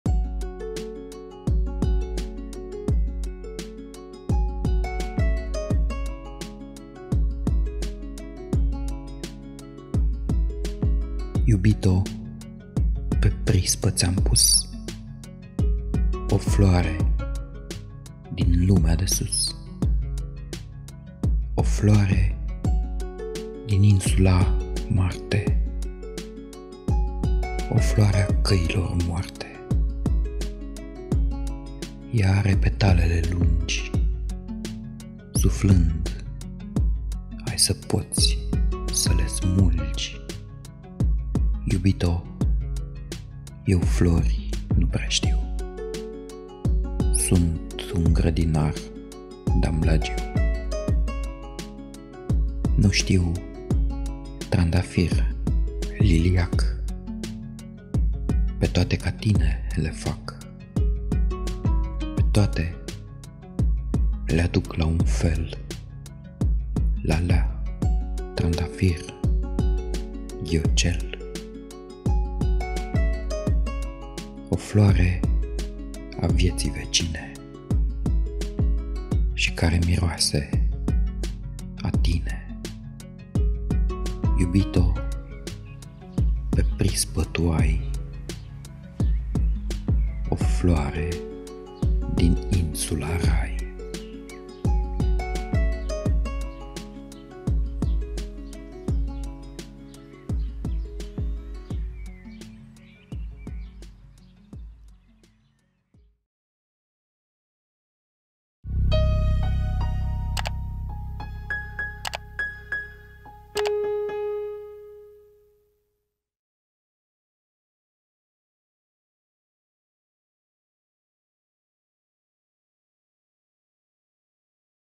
Aceeași floare - Adrian Păunescu
Iubito, pe prispă ți-am pus
O floare din lumea de sus,
O floare din insula Marte,
O floare a căilor moarte.

Ea are petalele lungi,
Suflând ai să poți să le smulgi,
Iubito, eu flori nu prea știu,
Sunt un grădinar damblagiu.

Nu știu - trandafir, liliac
Pe toate ca tine le fac,
Pe toate le-aduc la un fel
Lalea, trandafir, ghiocel.

O floare a vieții vecine
Și care miroase a tine.
Iubito, pe prispă tu ai
O floare din insula Rai.

Adrian Păunescu (n. 20 iulie 1943, Copăceni, județul Bălți, România, astăzi în Republica Moldova — d. 5 noiembrie 2010, București) a fost un autor, critic literar, eseist, director de reviste, poet, publicist, textier, scriitor, traducător și om politic român.